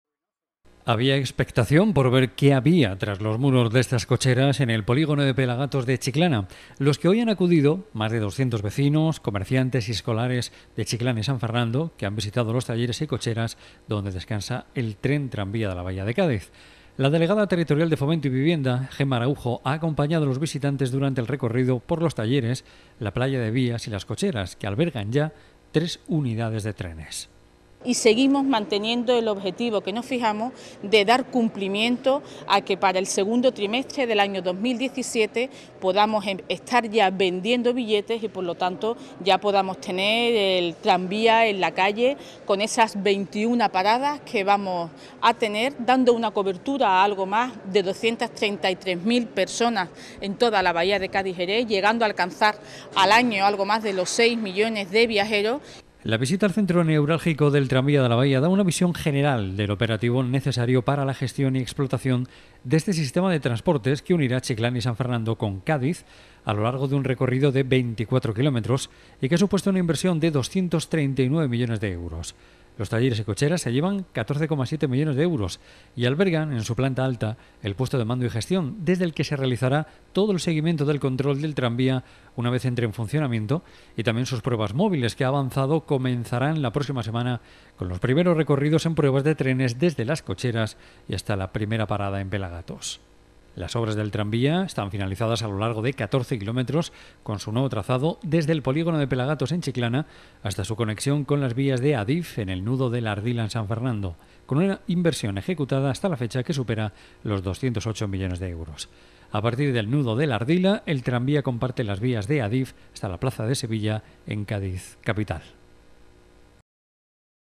PUERTAS ABIERTAS
A medidados de 2017 se estarán vendiendo billetes para poder coger el tren tranvía de la Bahía de Cádiz, según ha confirmado la delegada territorial de Fomento, Gema Araujo. Hoy ha estado visitando junto con otras doscientas personas las instalaciones que este transporte tiene en Pelagatos.